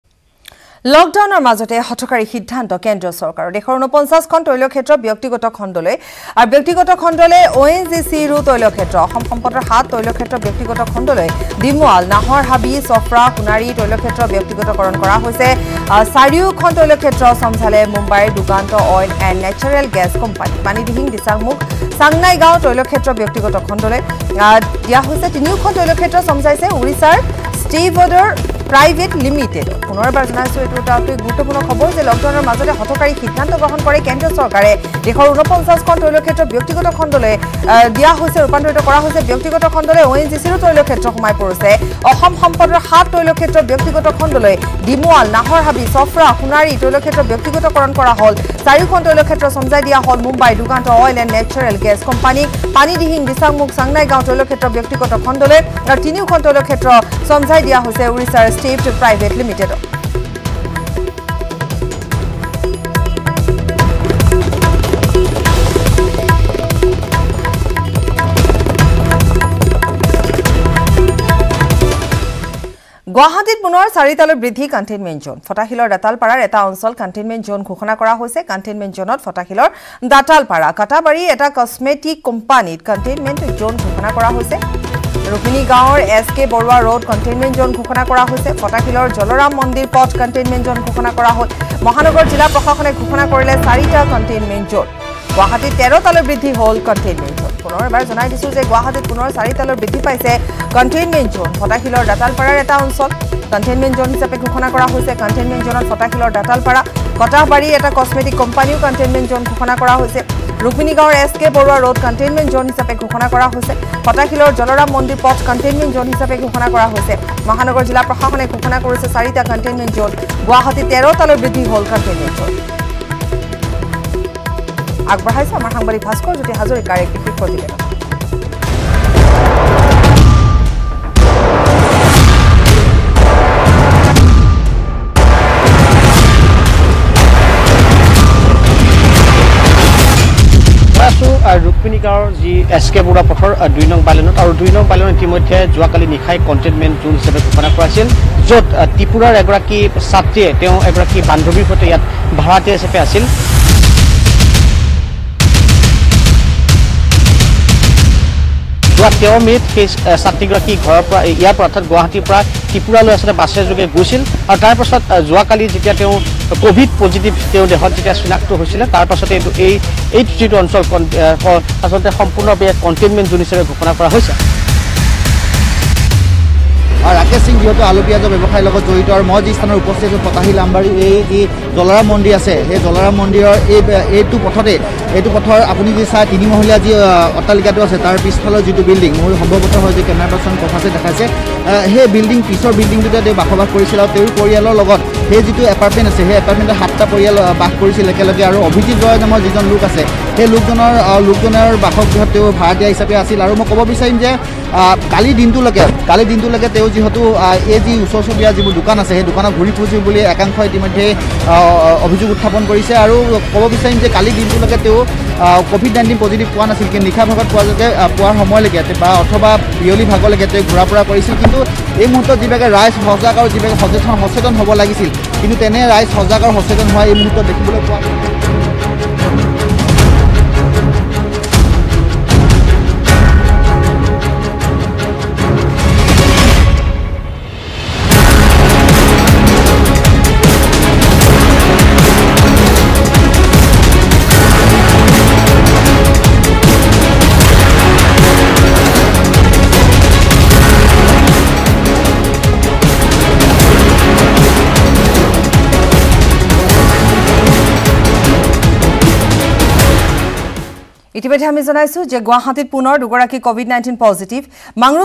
Lockdownৰ সময়তে চৰকাৰৰ হঠকাৰী সিদ্ধান্ত, দেশৰ ৮৯খন তৈলক্ষেত্ৰক কৰা হৈছে ব্যক্তিগতকৰণ
ব্যক্তিগতখণ্ডলৈ ONGCৰো তৈলক্ষেত্ৰ । অসম সম্পদৰ ৭খন তৈলক্ষেত্ৰ ব্যক্তিগতকৰণ । চাওঁক এই বিশেষ প্ৰতিবেদন ।